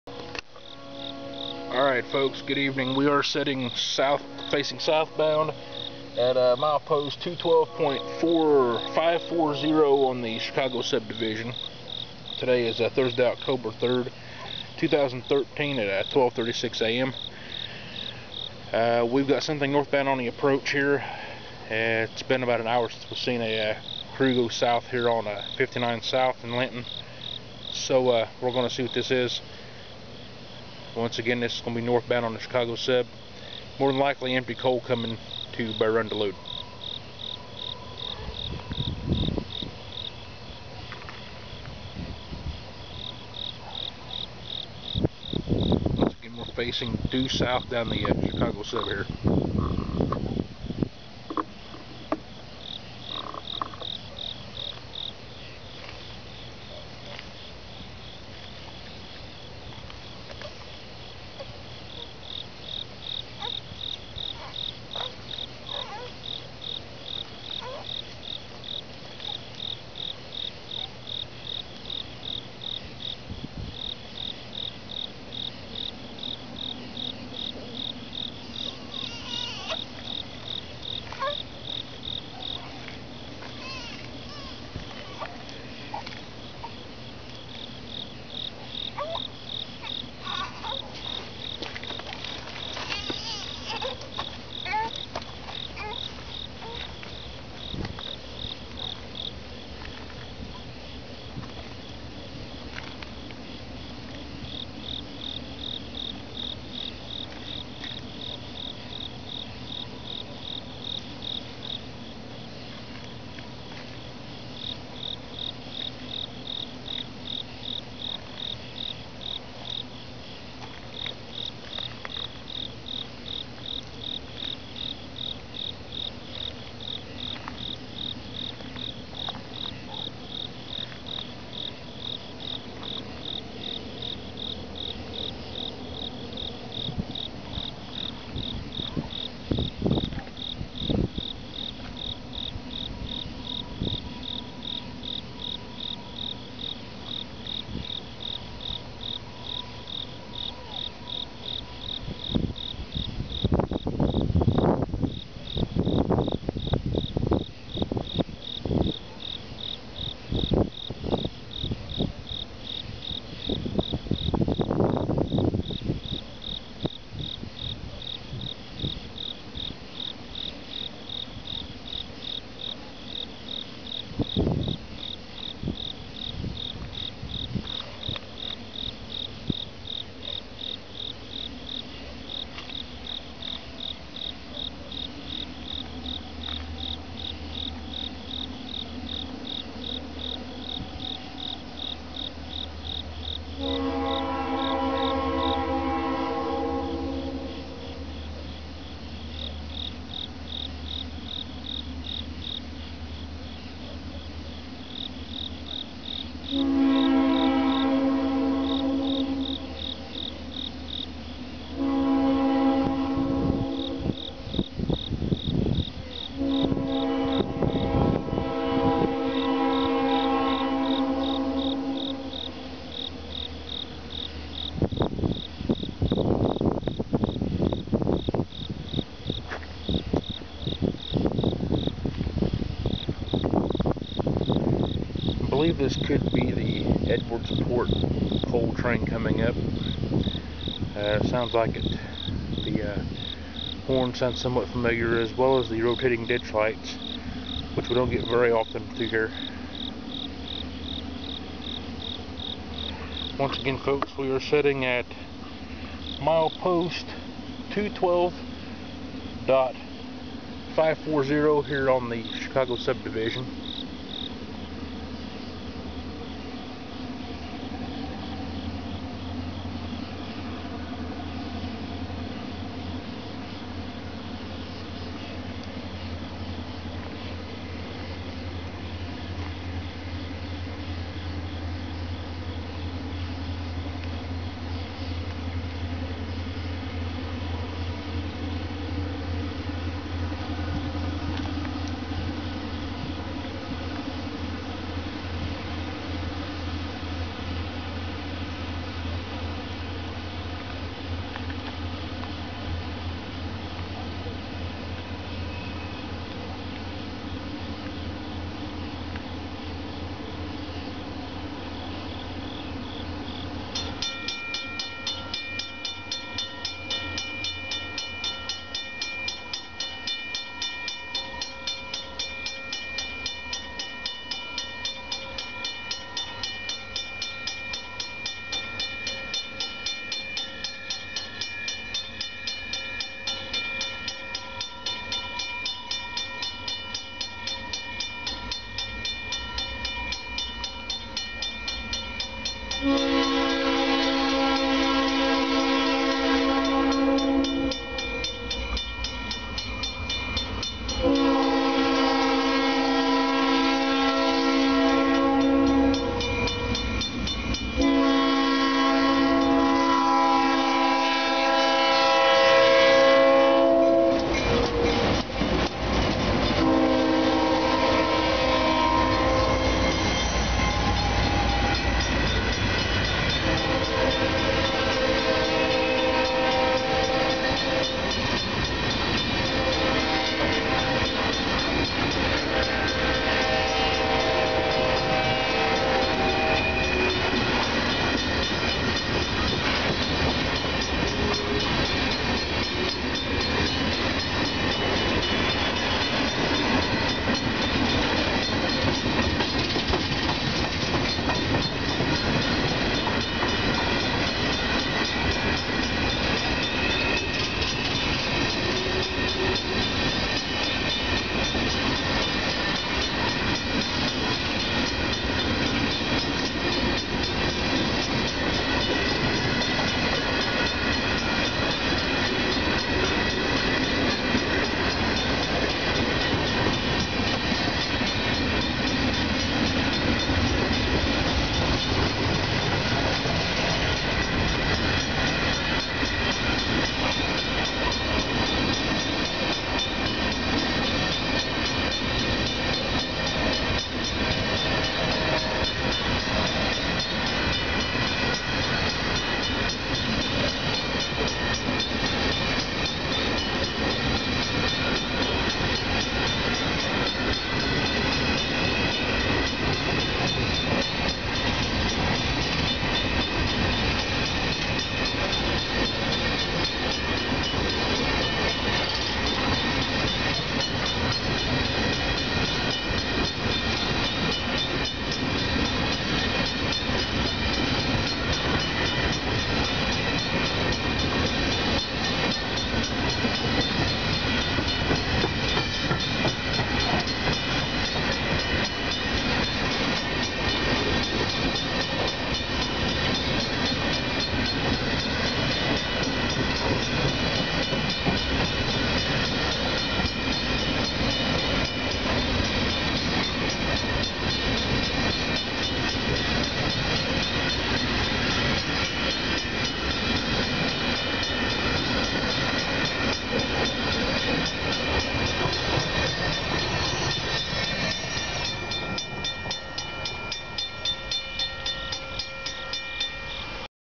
CIT 6054 leads empty coal,,Linton,IN 10-3-13
This was yet,another "Chance Encounter",this time with what is know as the "Edwardsport Coal Train",which runs at least once a week along the Chicago/Indianapolis sub-divisions of the INRD during its trips from the Duke Energy plant in Edwardsport,Indiana to Bear Run mine just south of Dugger,Indiana and then returning to the power plant.